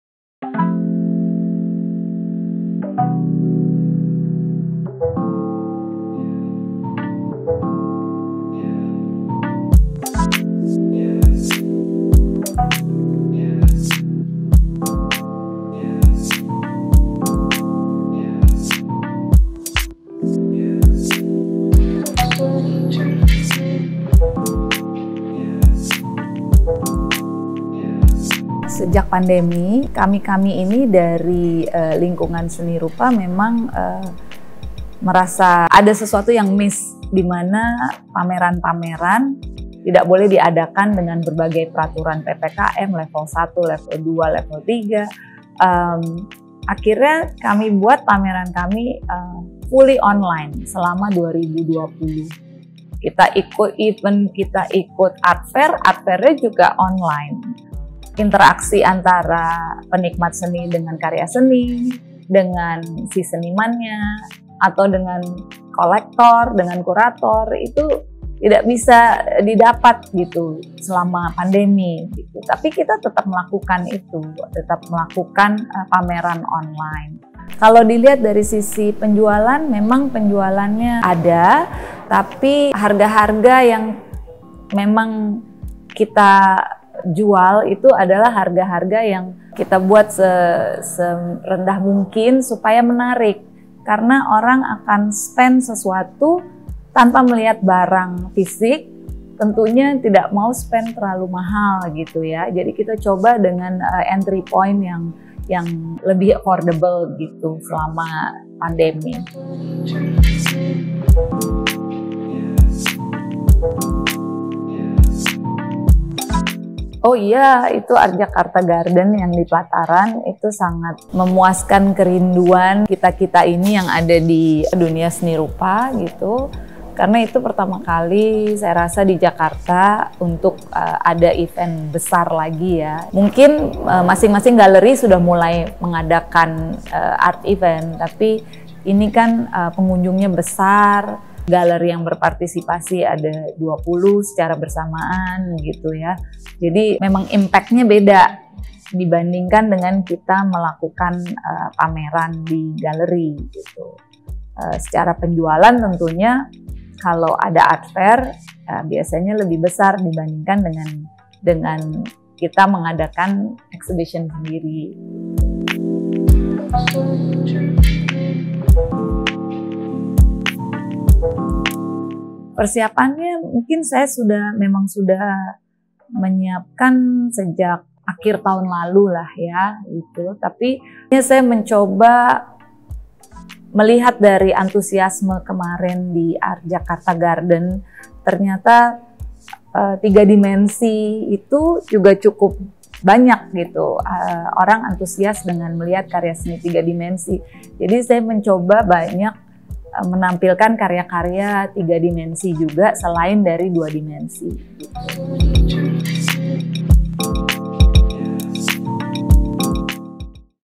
Interview Esti Nurjadin (Owner of D Gallerie): Dunia Seni Rupa di Masa Pandemi
(member of Vibiz Media Network)

Program : Interview Session

Kali ini Business Lounge Journal mengunjungi D Gallerie, sebuah galeri seni rupa di bilangan Kebayoran Baru, Jakarta. Berbincang dengan sang pemilik, Esti Nurjadin, ada berbagai hal menarik yang kami perbincangkan. Mulai dari bagaimana galeri ini berdiri, kabar dunia seni rupa dan galeri pada masa pandemi, hingga bagaimana seni rupa menjadi bagian dari sejarah bangsa kita tercinta.

Dalam video ini, Esti menuturkan bagaimana dunia seni rupa mengalami pukulan yang keras ketika memasuki masa pandemi. Namun semuanya dapat dilalui hingga semua kembali bangkit bersama-sama.